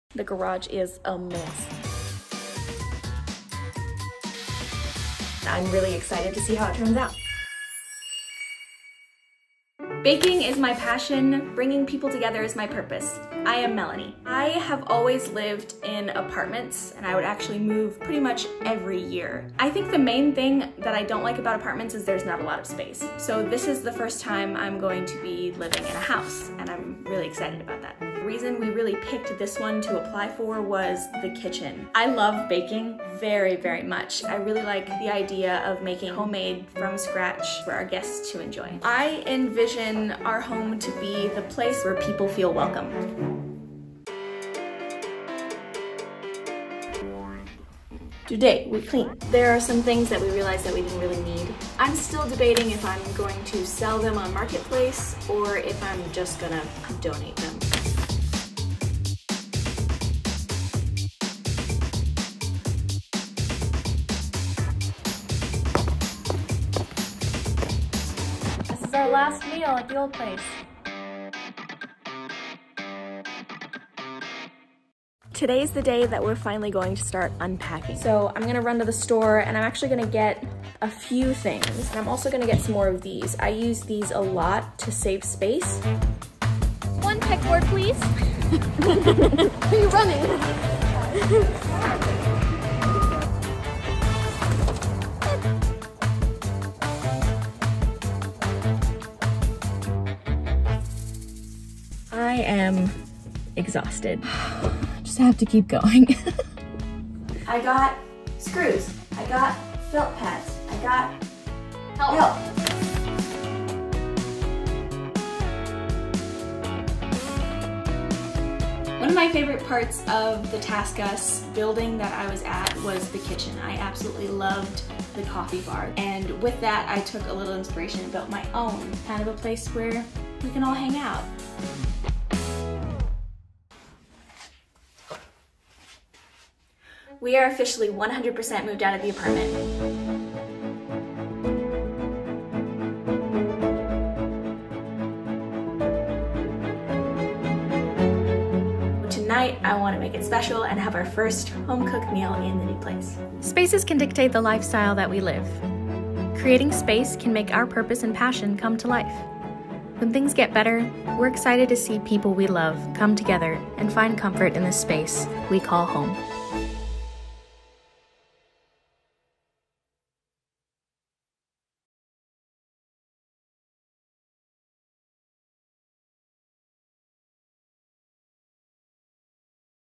Space Makeover
A dedicated space to nurture your passion? Sounds fun and exciting, but executing it can be a little challenging! Watch and learn as our teammate Melanie takes us on a journey in efficiently designing her RidiculouslyRefreshing home.